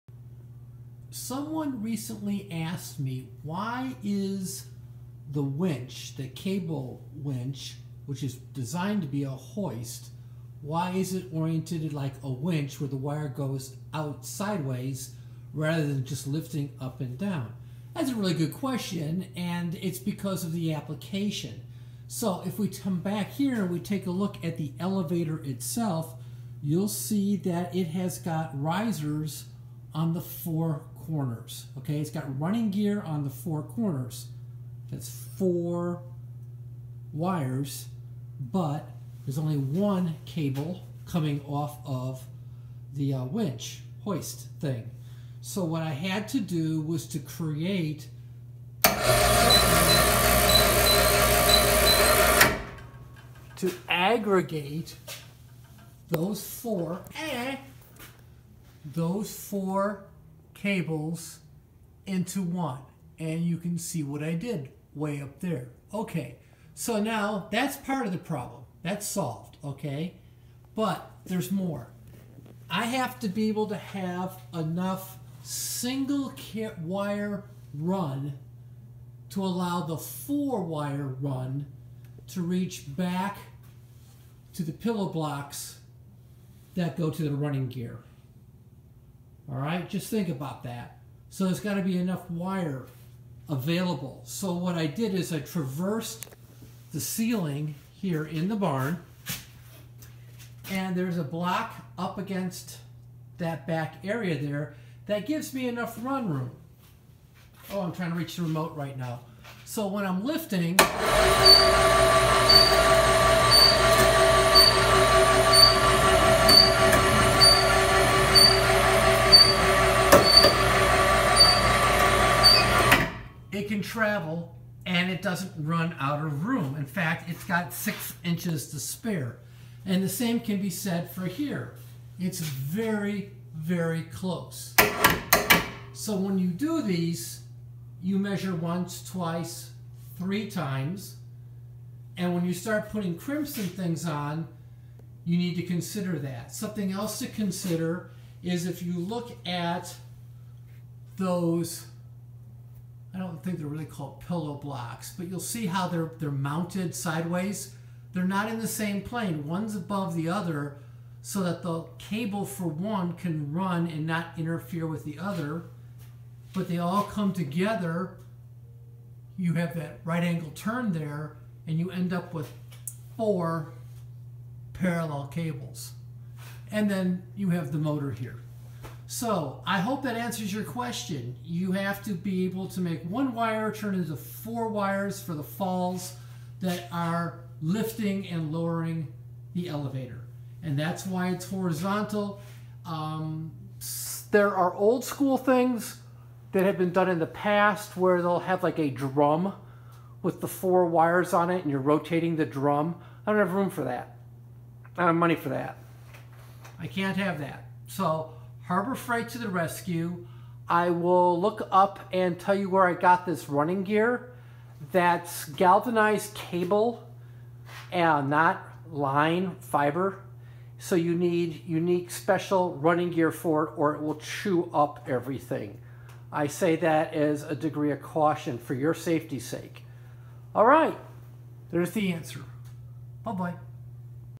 Horizontal cable to lift an elevator up? That's crazy talk!
A follower asked my why the lift cable from my winch is horizontal to lift the elevator up and down.  It was cold, but I wanted to record a quick video showing the motion of the running gear and how the falls to the elevator are combined into a single cable.  Also, the collected cables and single cable need enough travel room to give the elevator a full range of motion.

I bought my supplies from these two suppliers:

for the sheaves that are made for wire rope/cable, and have self-lubricating bushings.

for the cable, crimps, thimbles, and other bits. (Nice people that referred me to Block Division when they didn't have the parts that I needed.)

Do NOT get frugal on your hardware!  A falling elevator could be lethal.  Make sure that everything is load rated for the full lifting need, and that everything is rated for overhead lifting.